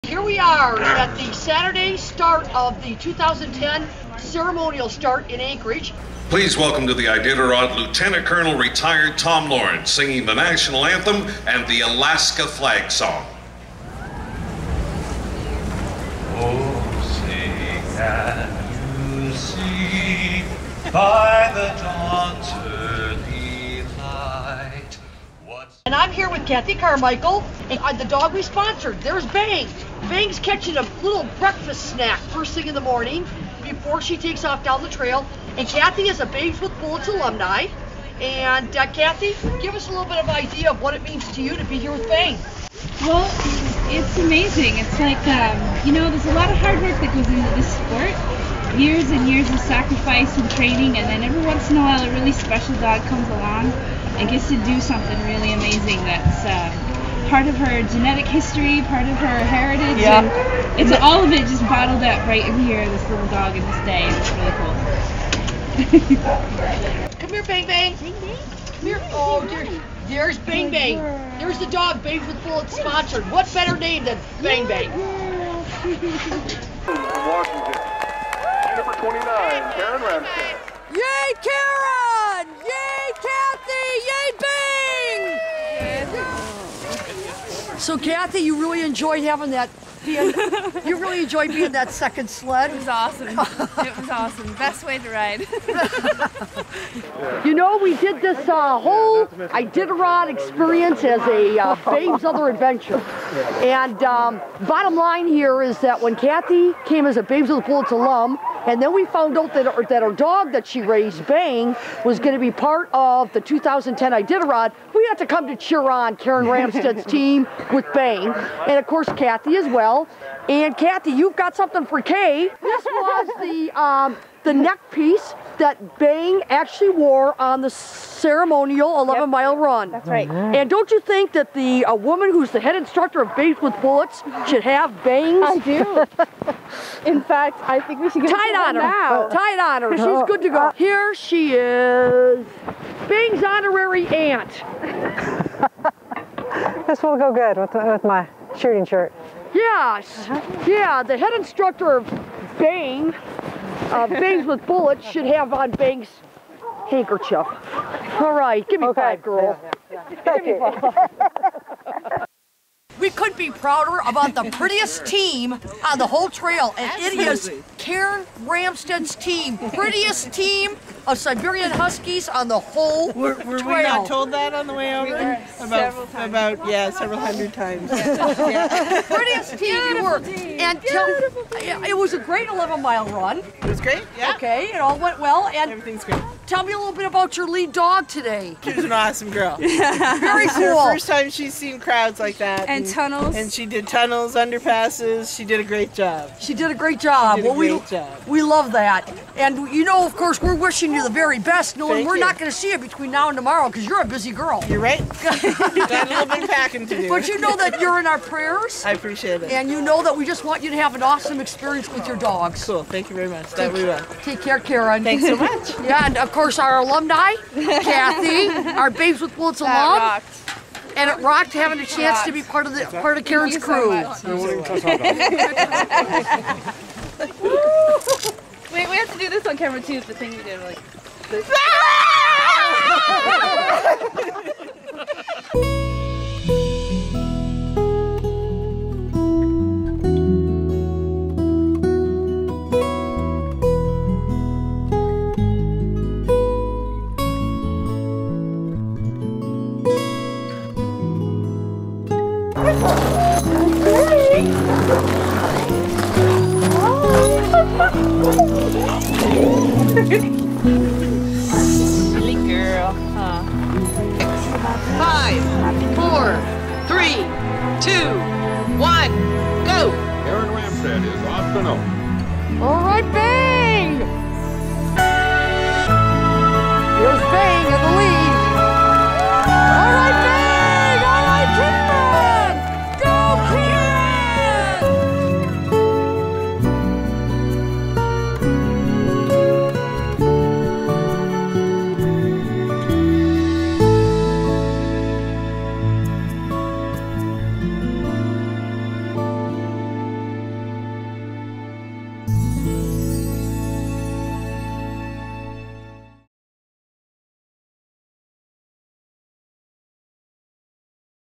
Bang - A Born Iditarod Leader
Kathy Carmichael, after having to get out of the dog mushing business in 2009, reunites with her very special dog Bang a year later.  Kathy's love and devotion to her dogs was rewarded by seeing Bang become the lead dog in Karen Ramstead's Iditarod team and being invited to share in the Iditarod's ceremonial festivities.  Please visit BabeswithBullets.com for other adventures. Filmed and edited by ProVideos4U.com located in Tucson, AZ.